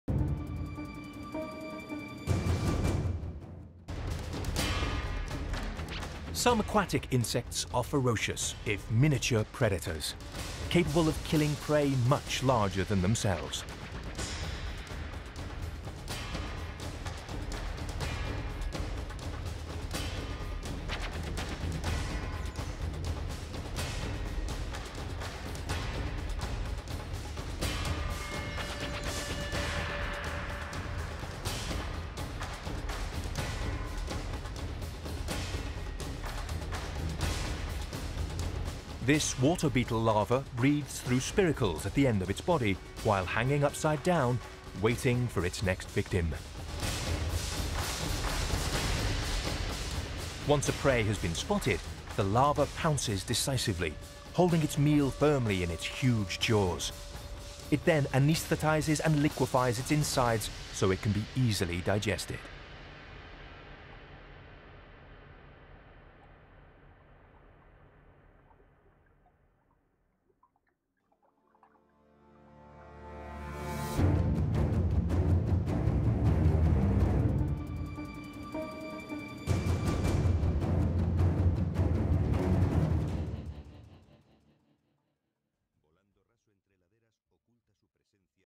Aquatic Vampire | Planet Doc Express
Some aquatic insects are ferocious – if miniature – predators, capable of killing prey much larger than themselves.
This water beetle larva breathes through spiracles at the end of its body while hanging upside down, waiting for its next victim.
Once a prey has been spotted, the larva pounces decisively, holding its meal firmly in its huge jaws.
It then anaesthetises and liquefies its insides so it can be easily ingested.